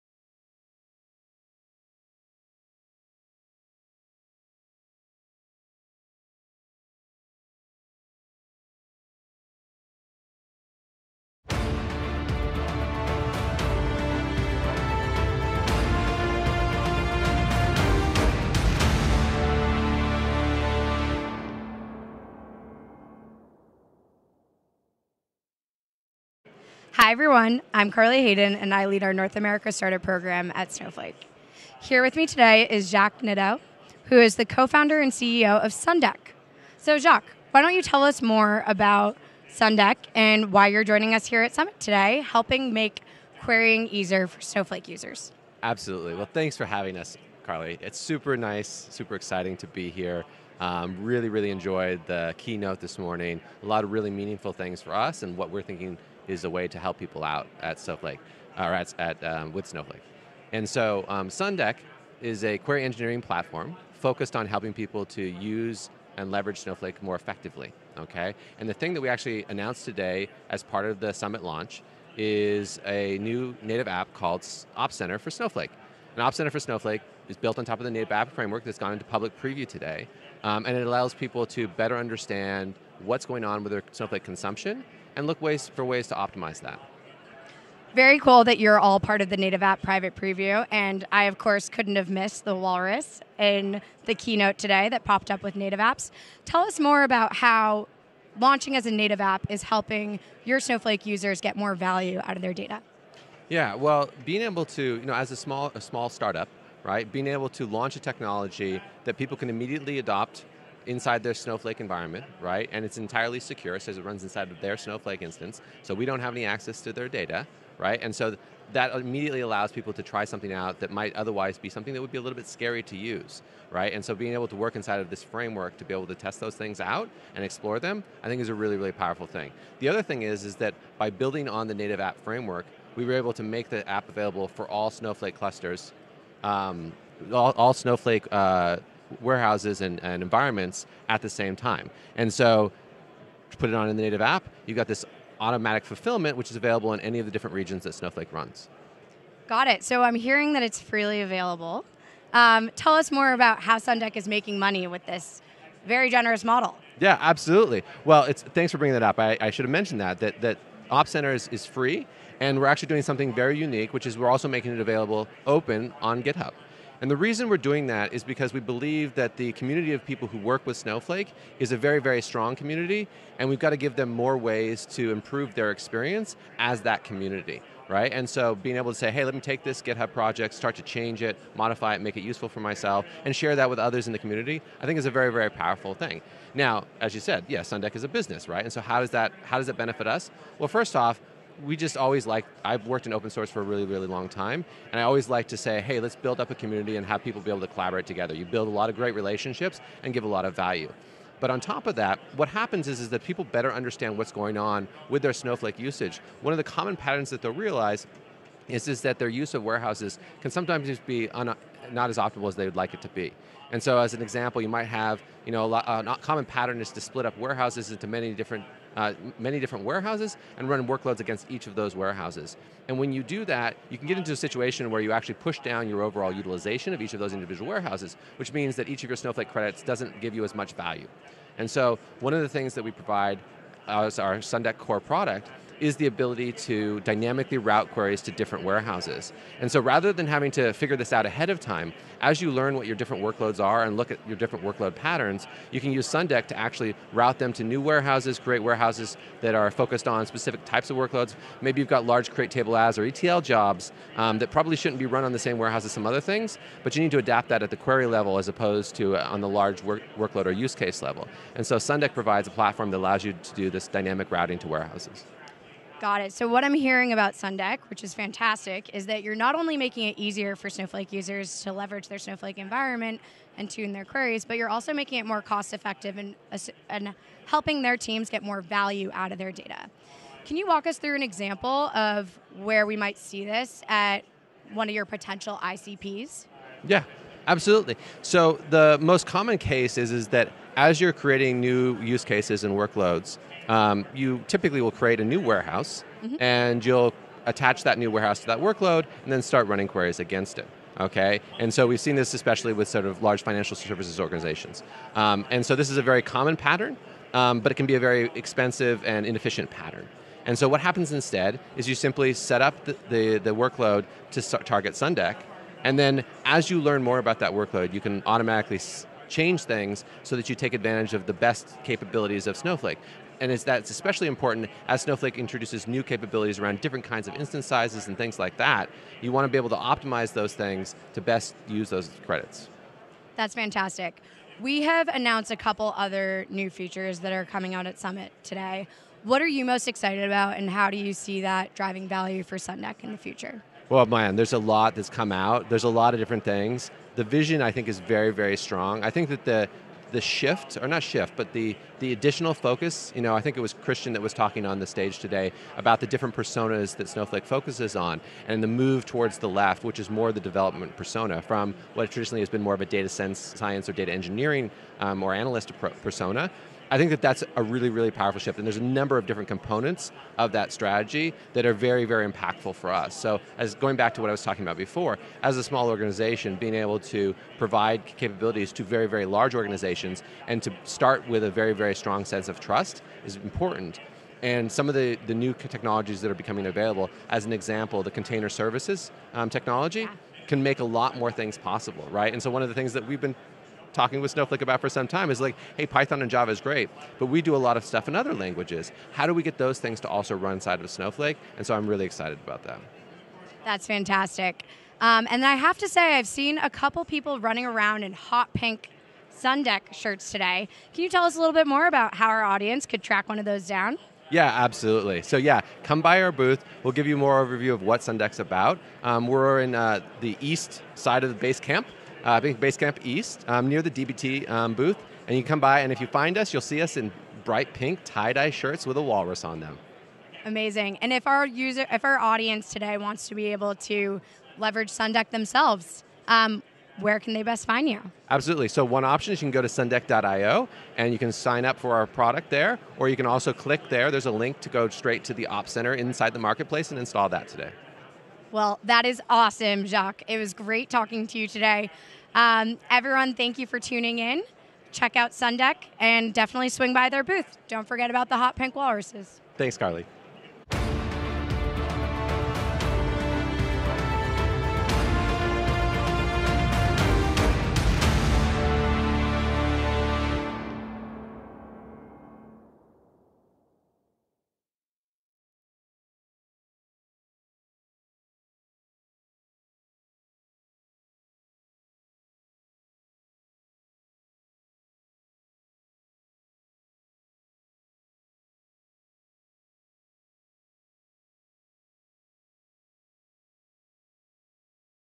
How Sundeck is Making Query Engineering Easier
Jacques Nadeau, CEO and Co-founder of Sundeck, talks with Snowflake Startup Program Lead Carly Hayden at Snowflake Summit about how Sundeck's query engineering platform allows analysts, data engineers, and DBAs to influence where, how, and what queries run on Snowflake. Sundeck is publicly (and freely) available for Snowflake users today via Snowflake's Native Application Framework. for more information.